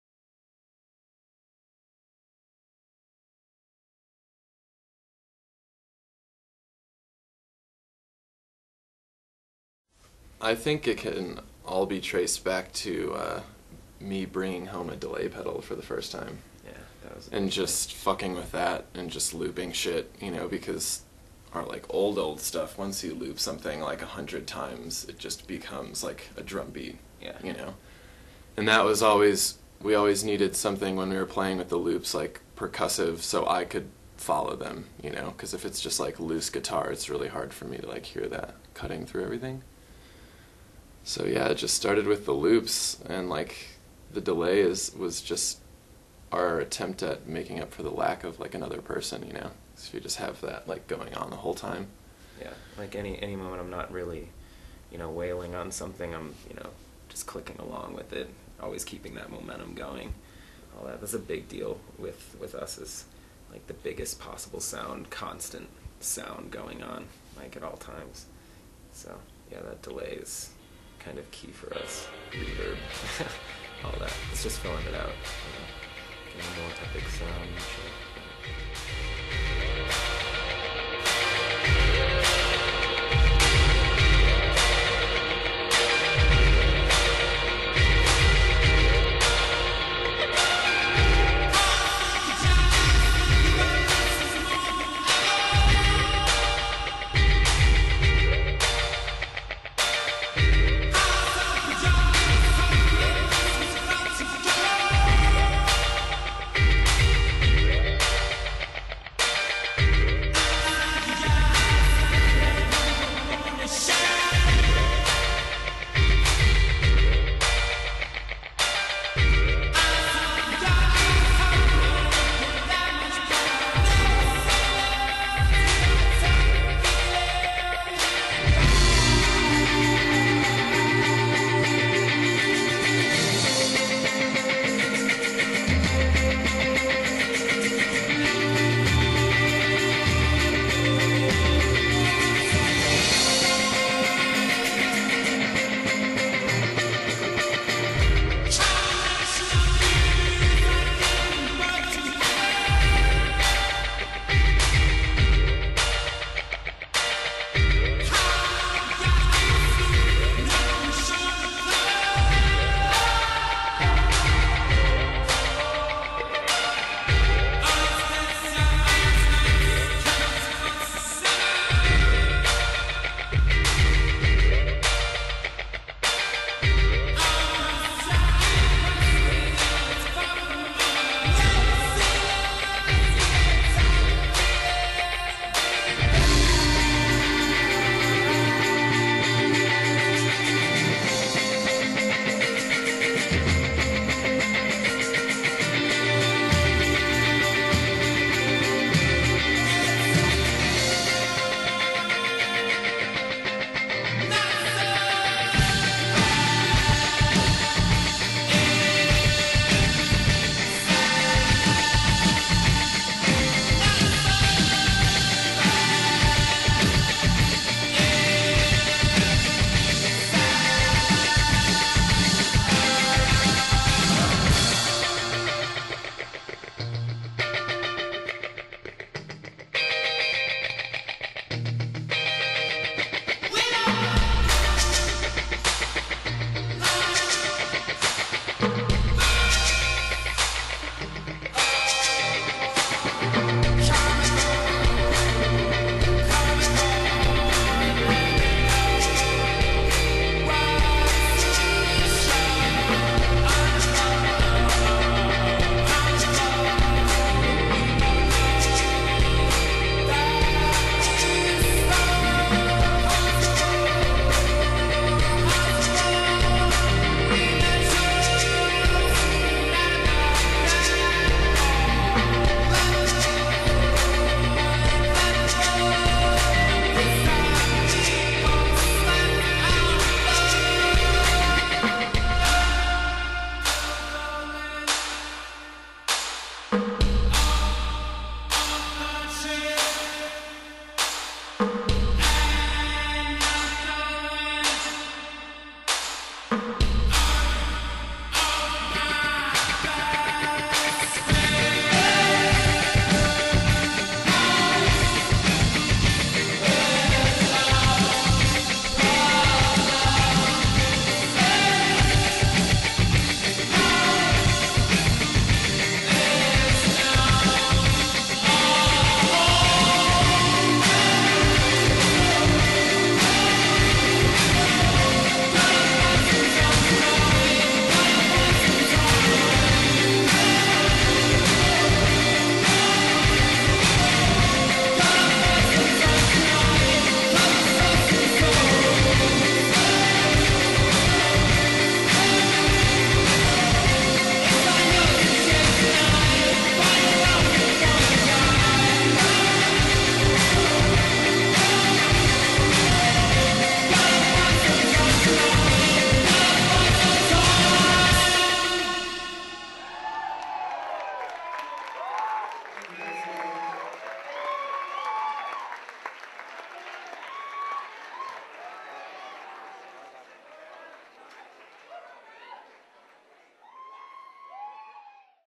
Gaunlet Hair - Speaker Snacks Film Series - Episode #6
The sixth installment of the Speaker Snacks Film Series features Gauntlet Hair. Originally from Chicago, Gauntlet Hair has found a new home in Denver and their musical talent thrives. Interview conducted by Tom Murphy. Filmed and edited by Dane Bernhardt.